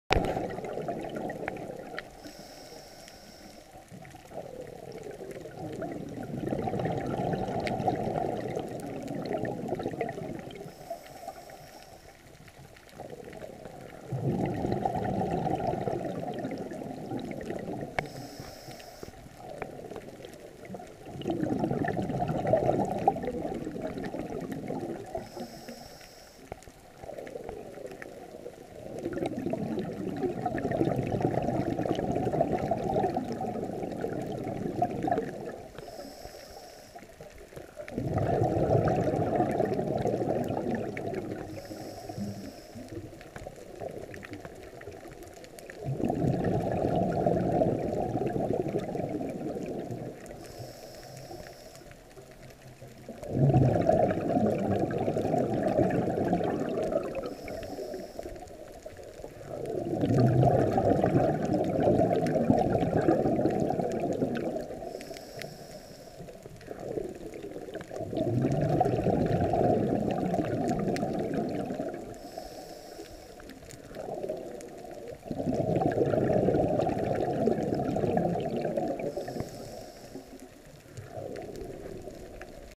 青苔小海龜
起流嘞!  牠在流頭，我在流尾。
而牠想順流離開開，就繞了我半圈，順流遊走嘞!